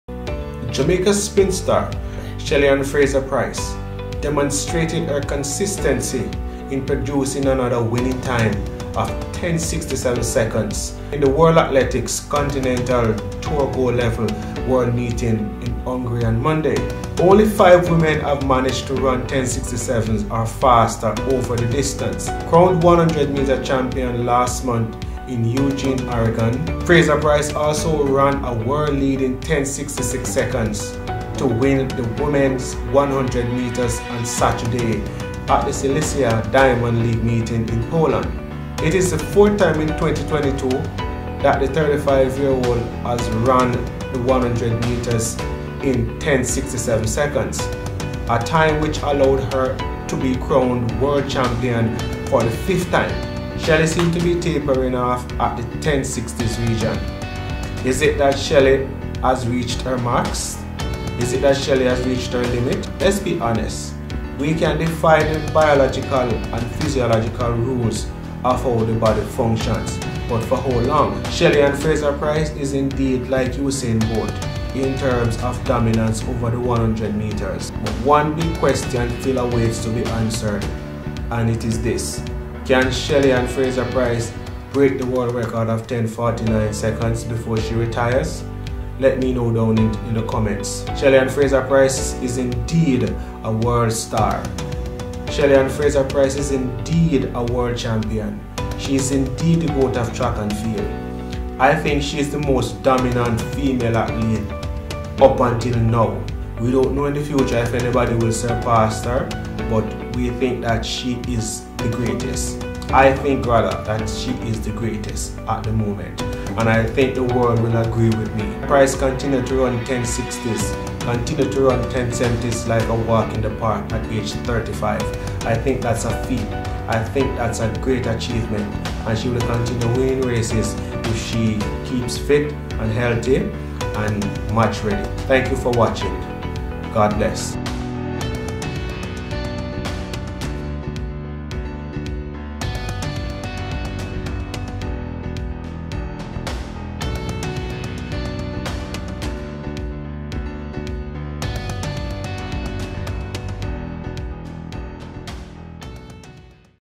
Shelly Ann Fraser Pryce One of the worlds greatest Track Stars. Can she run faster than 10.60's ?.
sports circle 345
Jamaica's sprint star Shelly-Ann Fraser-Pryce demonstrated her consistency in form with a winning time of 10.67sec in the World Athletics Continental Tour Gold Level Meeting in Szekesfehervarin, Hungary on Monday.

Crowned 100m world champion last month in Eugene, Oregon, Fraser-Pryce also ran a world-leading 10.66 seconds to win the women's 100 metres Saturday at the Silesia Diamond League meeting in Poland.

It is the fourth time in 2022 that the 35-year-old has run the 100m in 10.67sec, a time which allowed her to be crowned world champion for the fifth time.

Only five women have managed to run 10.67 or faster over the distance, including American world record holder, the late Florence Griffith-Joyner's 10.49 in 1988.

US vice-world champion Marvin Bracy dominated the men's 100m in 9.97sec, ahead of compatriot Trayvon Brommell (10.01).